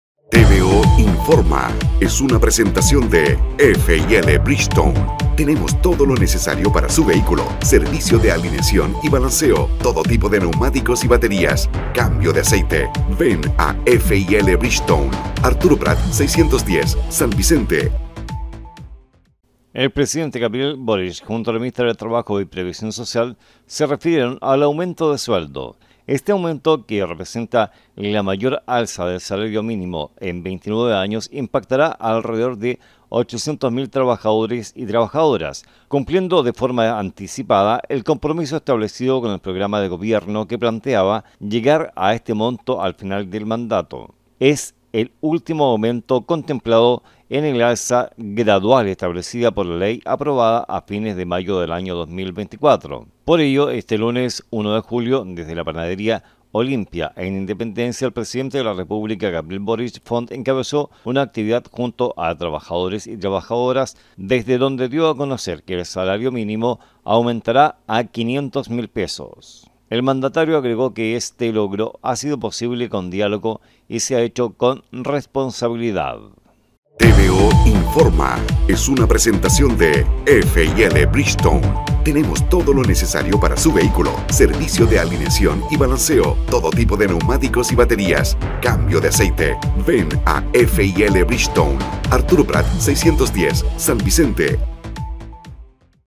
Aumento del salario mínimo en Chile
Desde el lunes 1 de julio de 2024, el sueldo mínimo en Chile es de $500 mil pesos.

Este aumento aplicará para al menos 800 mil trabajadores; y se trata del último ajuste a la renta mínima que se lleva de modo gradual desde mayo del 2023, cuando subió de $410 mil a $440 mil; y luego a $460 mil en septiembre del mismo año.